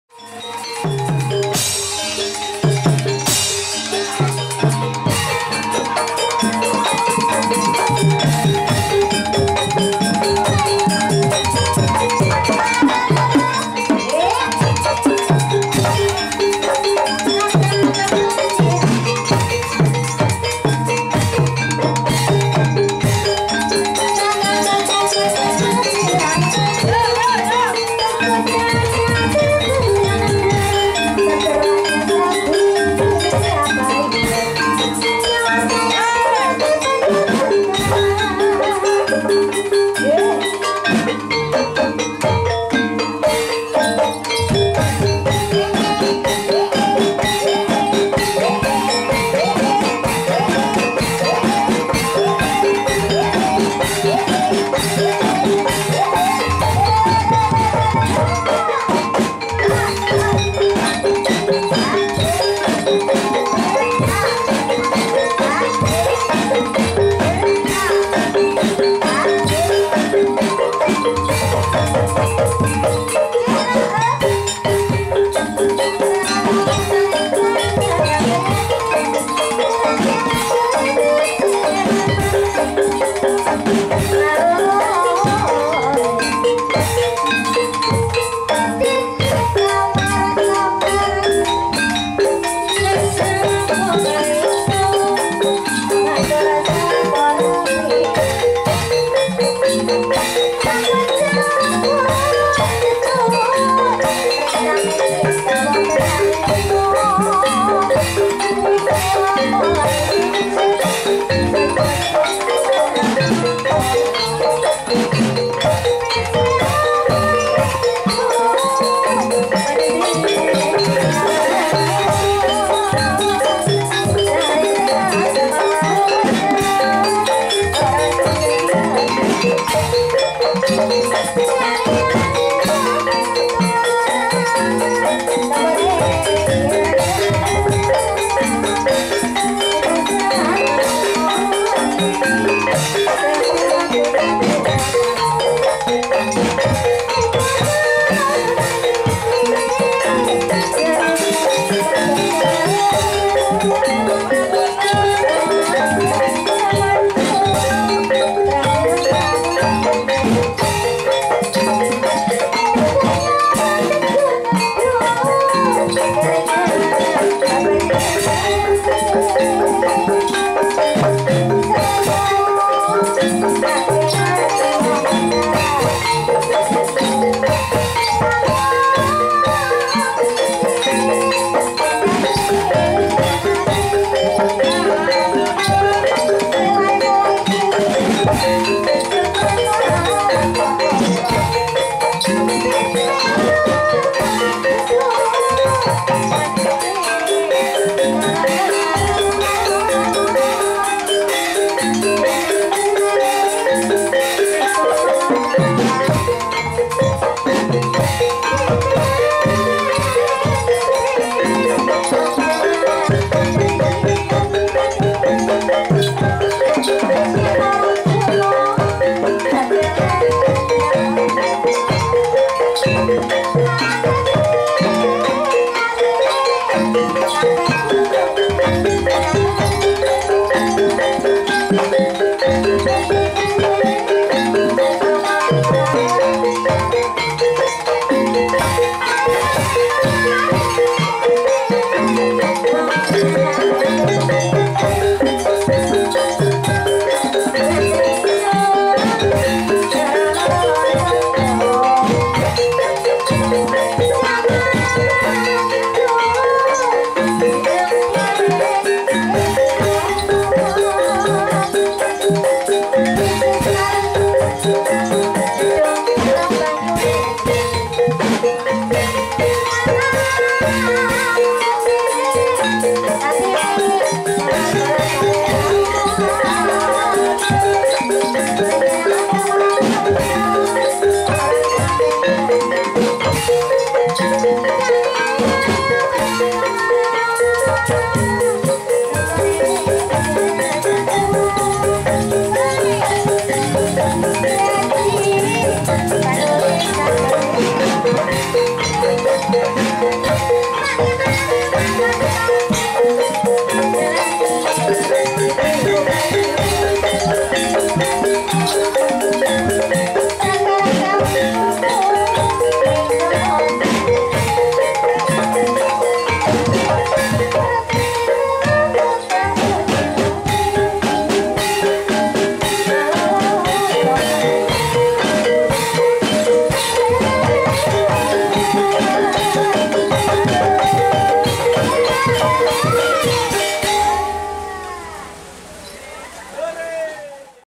TARI LENGGERAN BODORAN BENDRONG KULON LINA DAN BOLOT EBEG WAHYU CAKRA BUANA
penisihan sidanegara kedungreja 4 april 2021